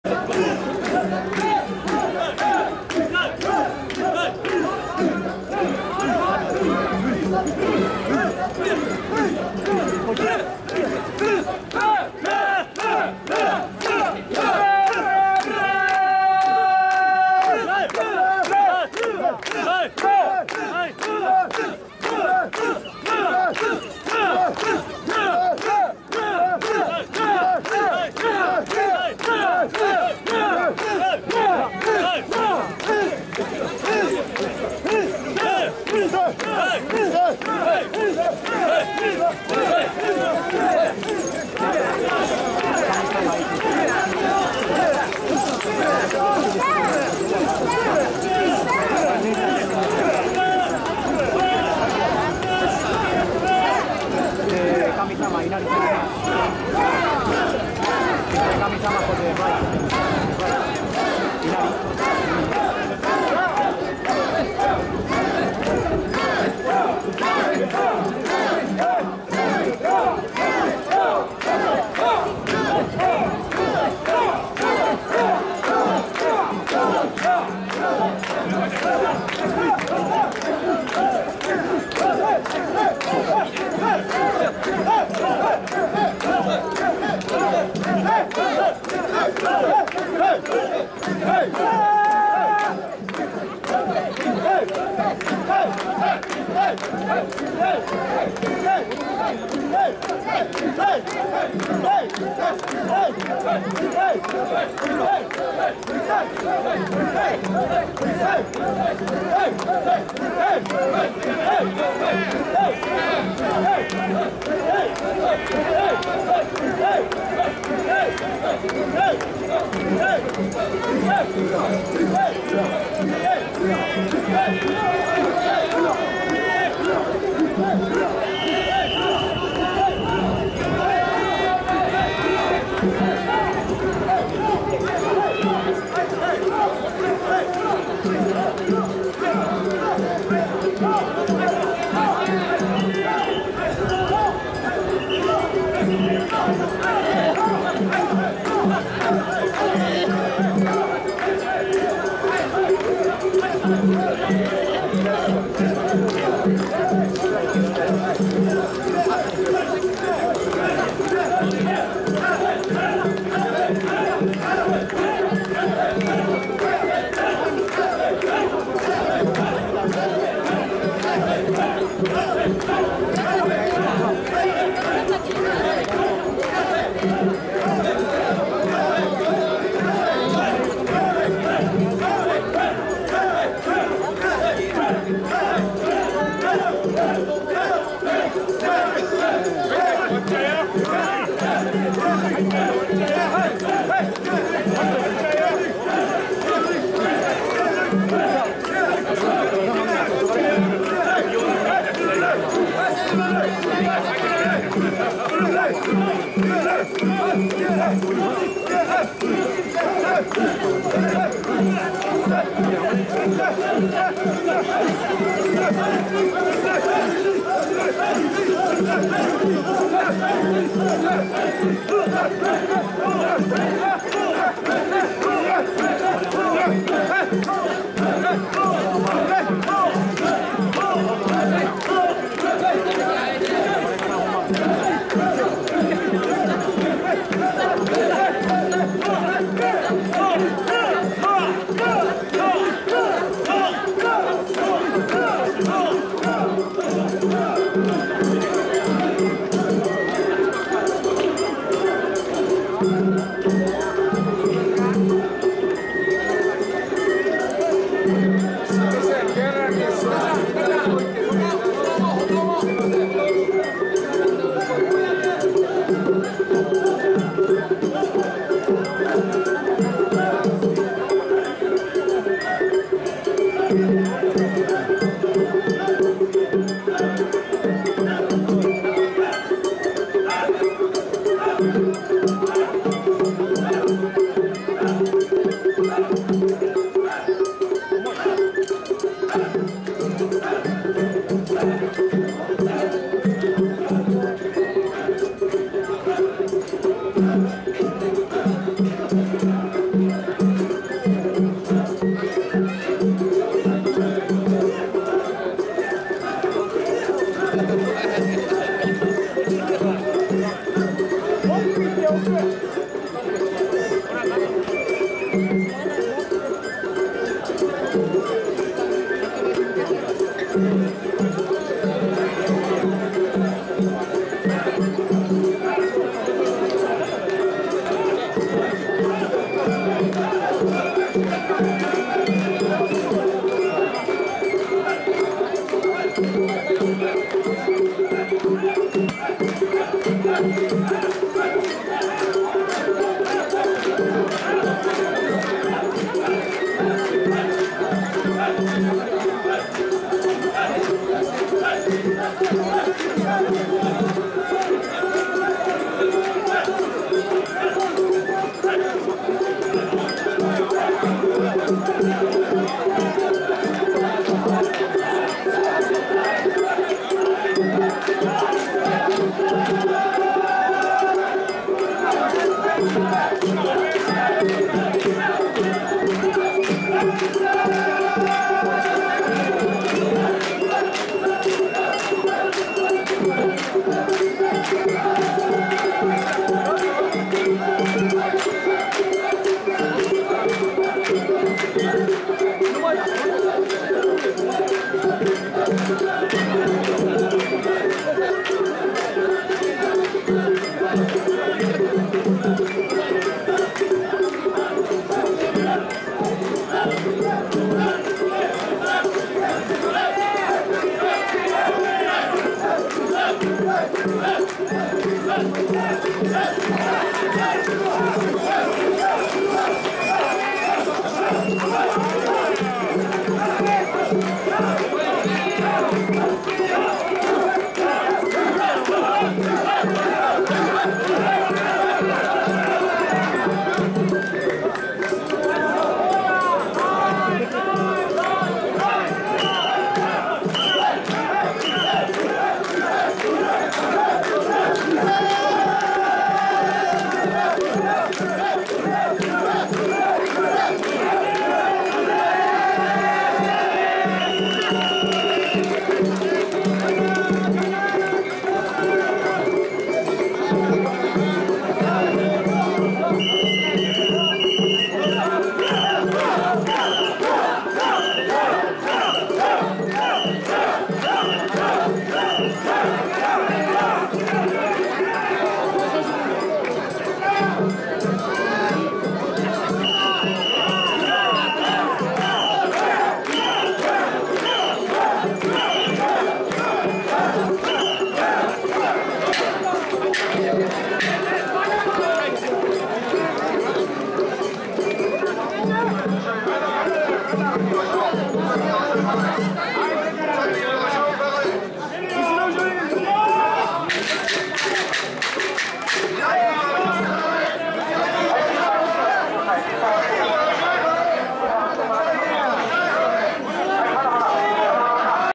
平成28年 三崎稲荷神社 例大祭 宮神輿町内渡御 三崎流ﾊﾞﾄﾙ担ぎ｡
三崎稲荷神社　例大祭　二年一度 宮神輿町内渡御です。
今日は 担ぎ手の皆さん大変お疲れ様でした。
本日は　宮出し～宮入り迄撮影で御座います、立派な　宮神輿で御座います。　色々とどうもありがとうございました。

三崎稲荷の氏子町は水道橋駅南から地下鉄神保町駅までの、
神田三崎町、三崎町一丁目、西神田、西神田三、神保町三丁目、
神西、神保町一丁目北部、一神、北神の九町会。
 最後の町会の神三町会が中通りで宮入道中に受け渡す。

 地名の由来。　三崎町という町名が誕生したのは明治５年（1872）、江戸に幕府が開かれる前に、

 この地にあった三崎村が町名の由来です。
 江戸が開発されるまで現在の大手町から日比谷や新橋周辺には日比谷入江と言われる遠浅の海が広がっていた。
三崎村は日比谷入江に突き出した「ミサキ（岬）」だったためこの名が付いたといわれる。
日本と言えば、神輿です！！ CUBA & JAPON.。　編集無し　其のまま RAW video （生）　Sony Full AVCHD (HD)　by picua。